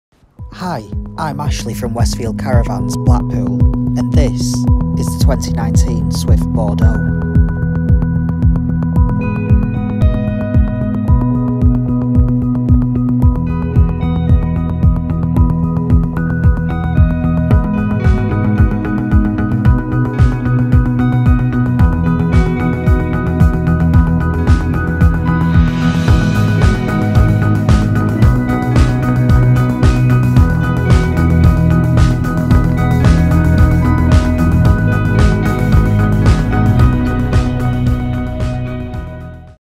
Westfield Caravans - 2019 Swift Bordeaux 33x12 KEN0274347
The well appointed and stylishly furnished Bordeaux range with comfortable freestanding sofas, dining table and chairs provides exceptional value for money. The "Chambray" soft furnishings scheme in coffee, yellow ochre and neutral tones mixes floral patterns with plaid accents for a homely feel

Please note - library images shown are of the two bedroom variant.

Delivery, Siting and Connections are not included. Please ask a member of the sales team for further information.